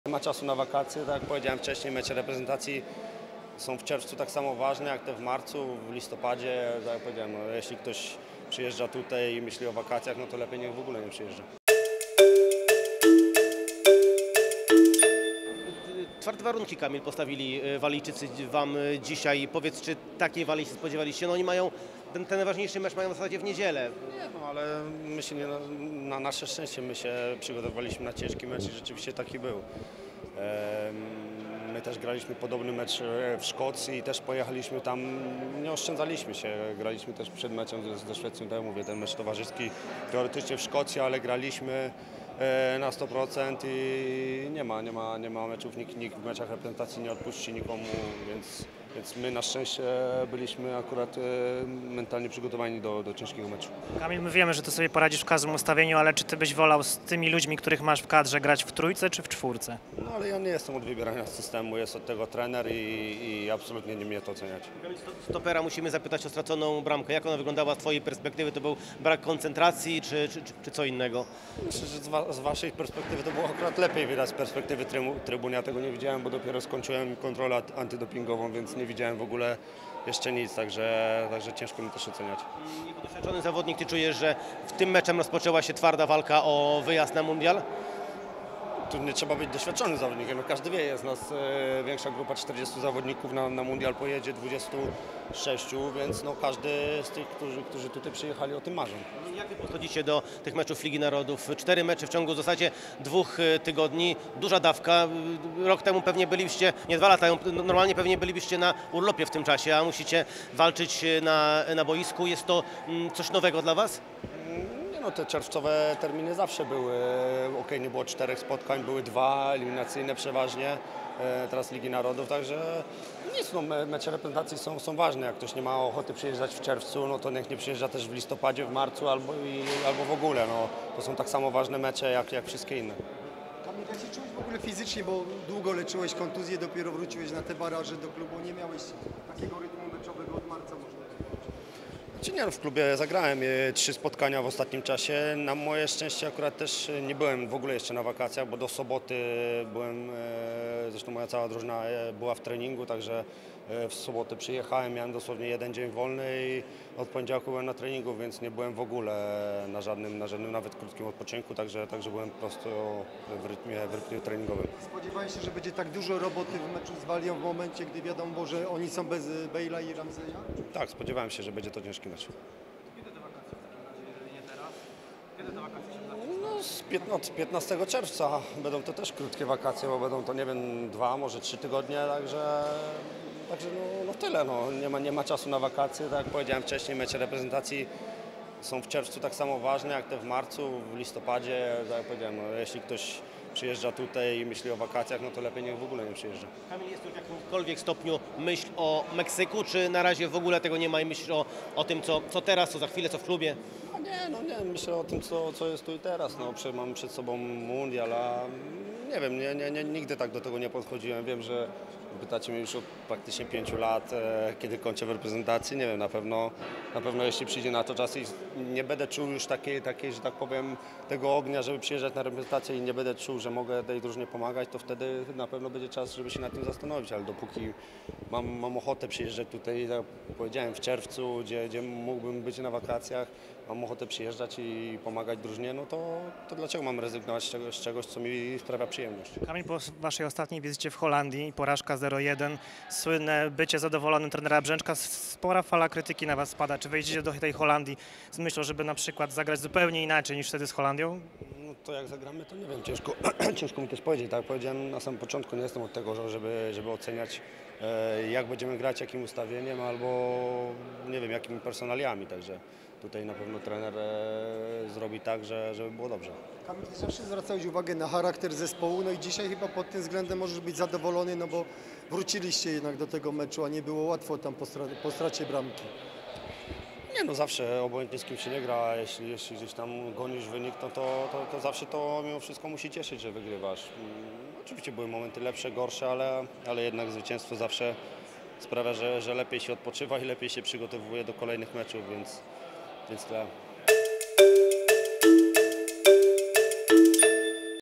Pytanie WKURZYŁO Kamila Glika: Jak ktoś nie ma OCHOTY...
Reprezentacja Polski udanie rozpoczęła zmagania w Lidze Narodów. Wprawdzie Biało-czerwoni przegrywali, ale zdołali się podnieść i pokonali 2:1 Walię w 1. kolejce tych rozgrywek. O sukcesie Polaków zadecydował duet rezerwowych. Bramki zdobyli Jakub Kamiński i Karol Świderski. Tak lider obrony polskiej kadry Kamil Glik w rozmowie z naszym portalem ocenił spotkanie we Wrocławiu. Powiedział także, jak widzi swoją przyszłość w drużynie narodowej.